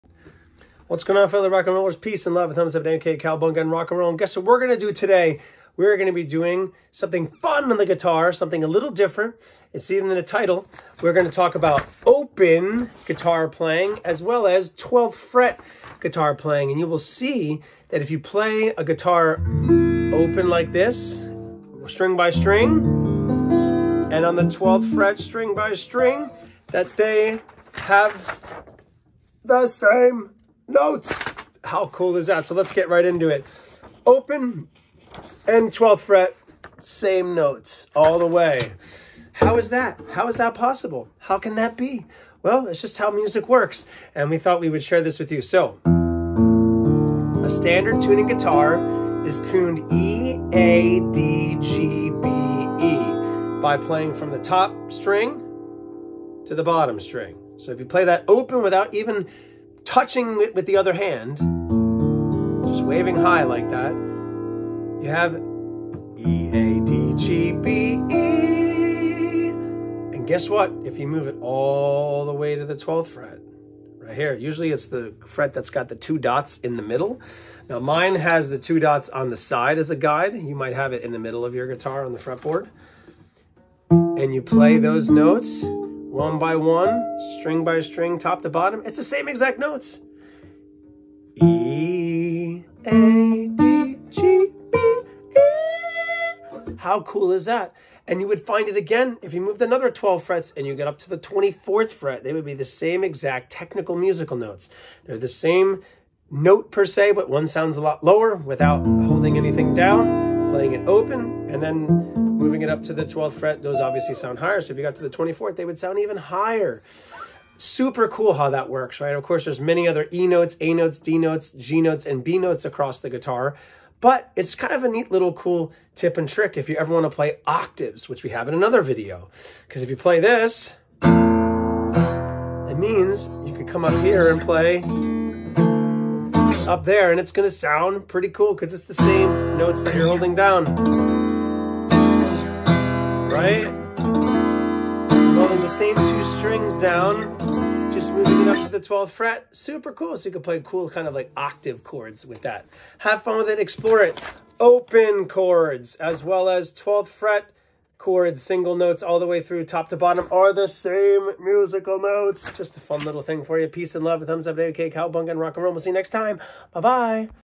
Did YOU Know OPEN Strings and 12th FRET Strings on GUITAR Are the SAME Notes? (Key of Awesome Music)
Did YOU Know OPEN Guitar STRINGS are the SAME NOTES on the 12th FRET?

Thank you for your support and enjoy!